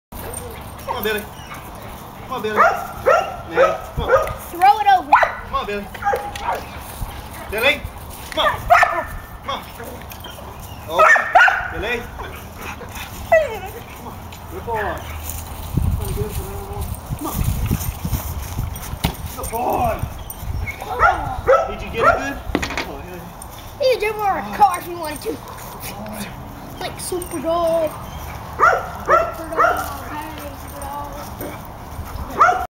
top guns bad boy billy1
malanois  jumps hurdle, this is before training at LA-K9